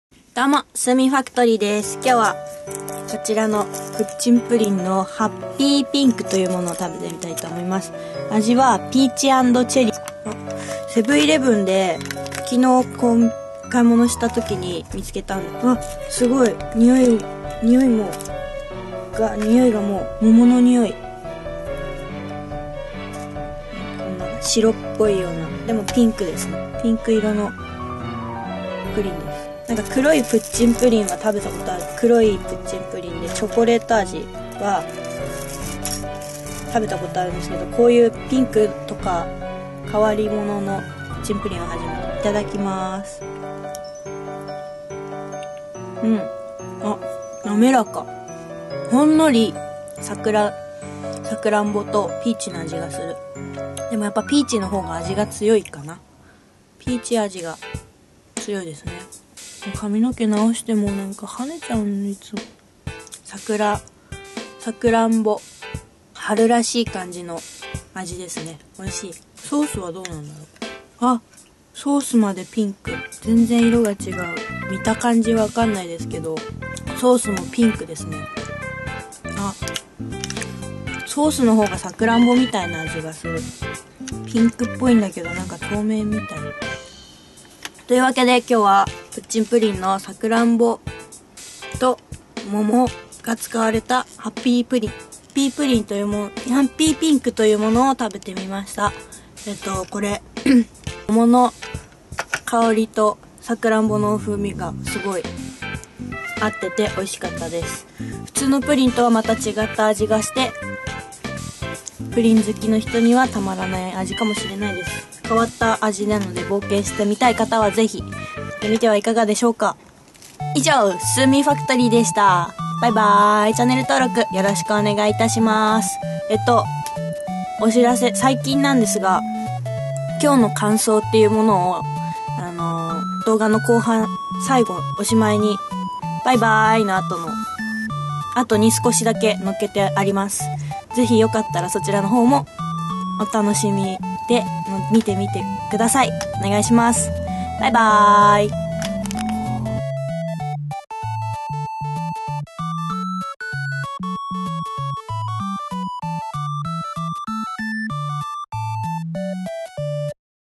プッチンプリンで春を先取り！！ハッピーピンク食べてみた。
セブンイレブンで買い物をしているときに発見したピンクのプッチンプリンです！！
味、見た目で春を先取りした気分で美味しくいただきました。

LAQでタイムアタックや商品紹介、実験、○○やってみた！！などいろんなことに挑戦中・・・